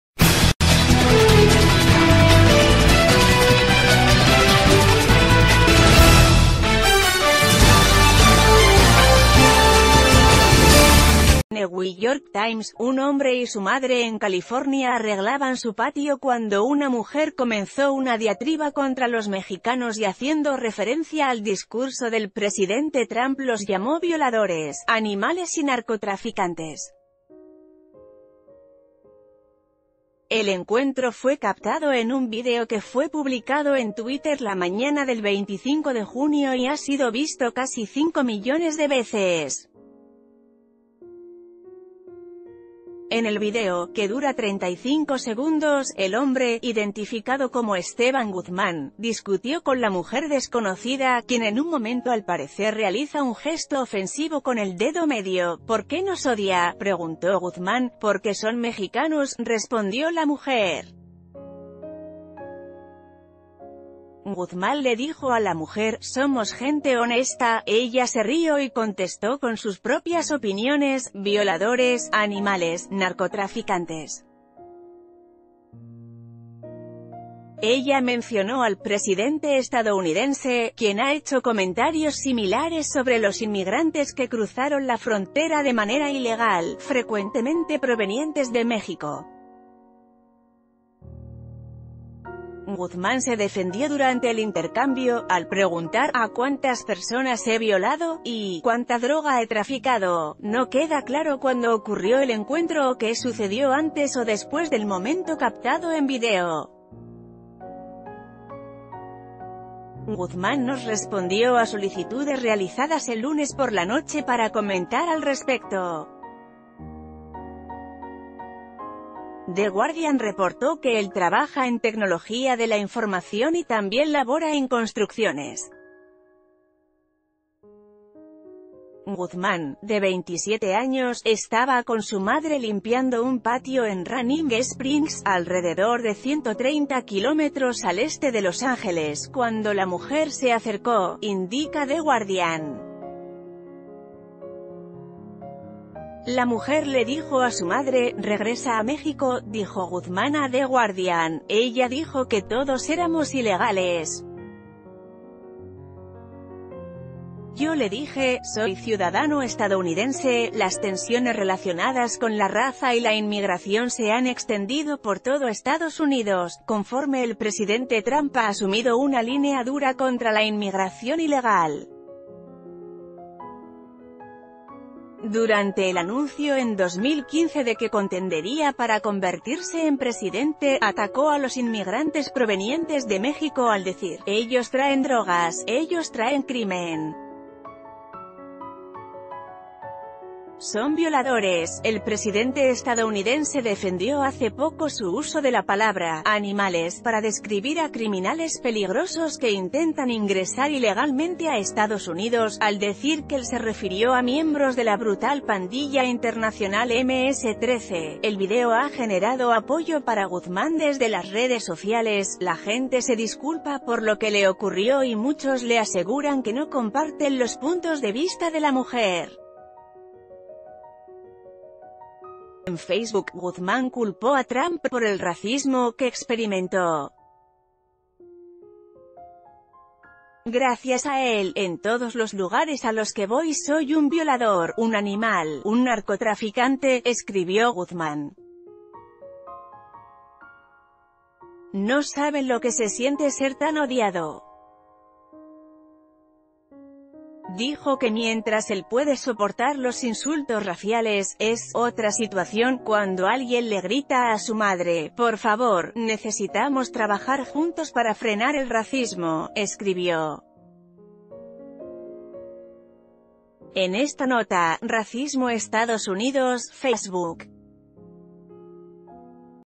‘¿Por qué nos odia?’, le preguntó; ‘Porque son mexicanos’, respondió ella
Newy York Times.- Un hombre y su madre en California arreglaban su patio cuando una mujer comenzó una diatriba contra los mexicanos y haciendo referencia al discurso del presidente Trump los llamó violadores, animales y narcotraficantes. El encuentro fue captado en un video que fue publicado en Twit...